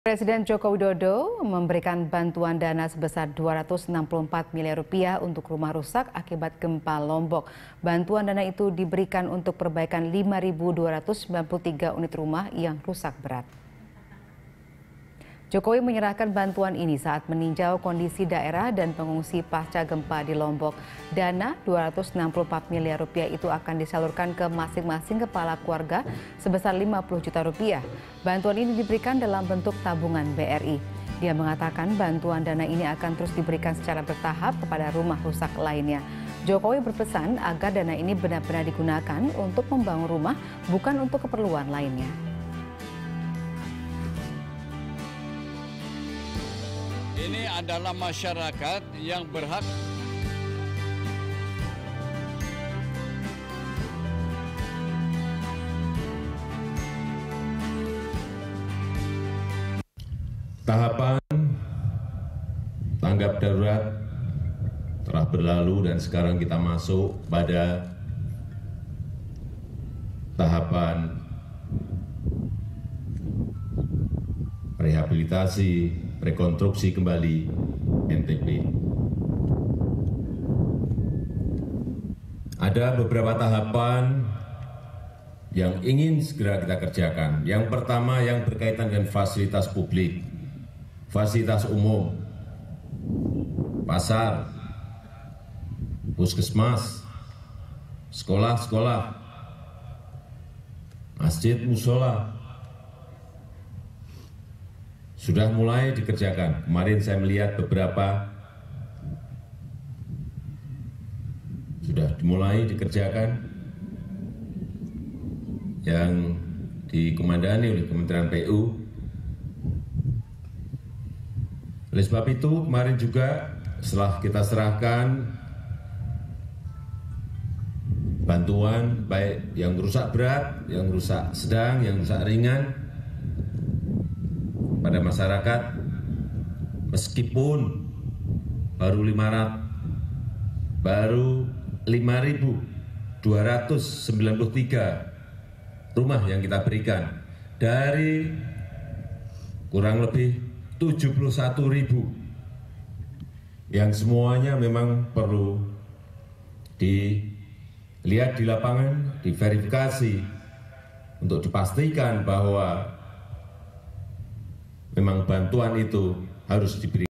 Presiden Salurkan Dana untuk Pembangunan Rumah di Lombok
Presiden Jokowi memberikan bantuan dana sebesar Rp 264 miliar untuk rumah rusak akibat gempa Lombok, NTB, Senin (3/9/2018). Bantuan dana diberikan untuk perbaikan 5.293 unit rumah yang rusak berat.

Facebook.com/BeritaSatuTV
Youtube.com/BeritaSatu